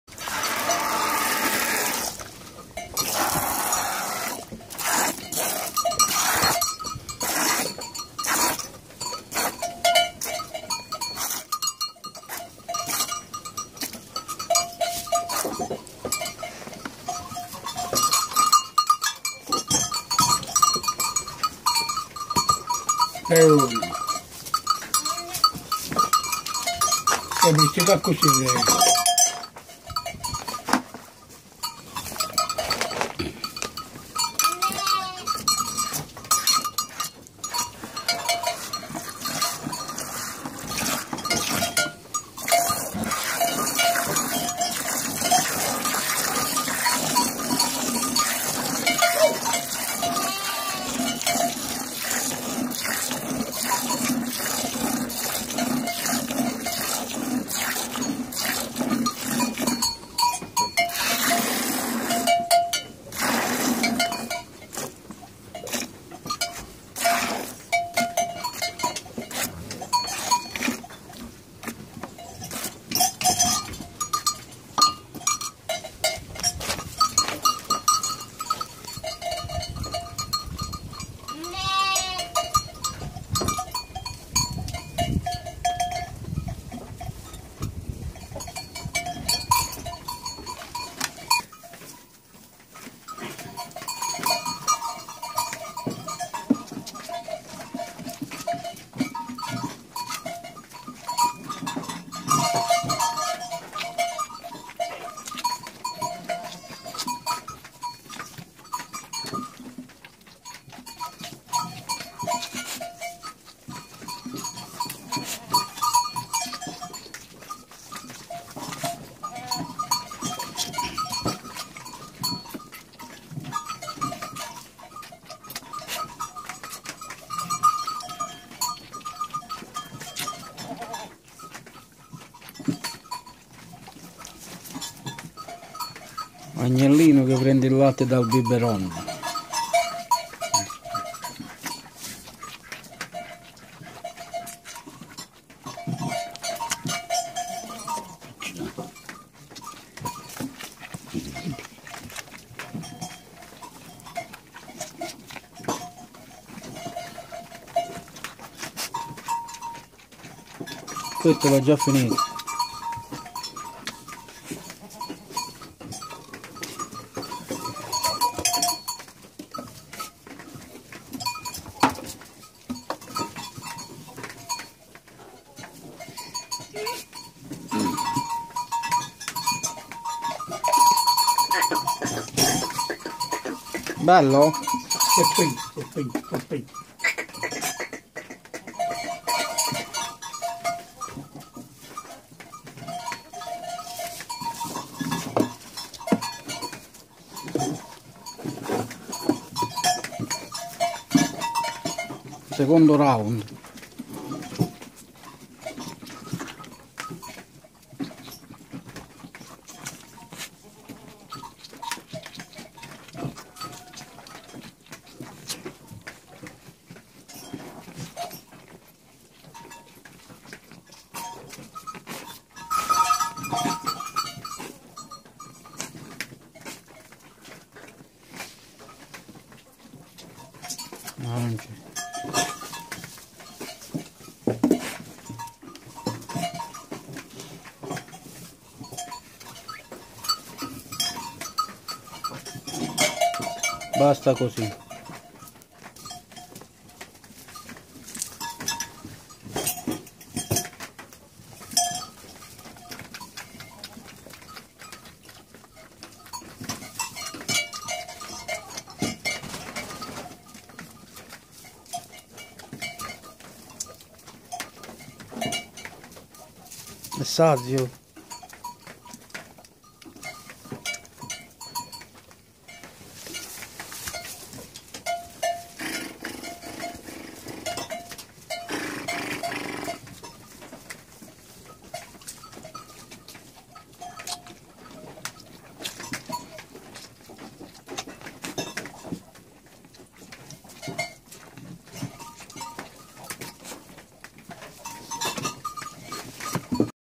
Mungere il latte delle caprette, per farlo bere col biberon all'agnellino
Mungitura di due caprette, lo stesso latte viene messo in una bottiglia con il biberò e dato da bere a un orfanello di agnellino. La mamma è morta quando lo ha partorito Video di alcuni mesi fa per chi non lo ha visto, e per i nuovi iscritti.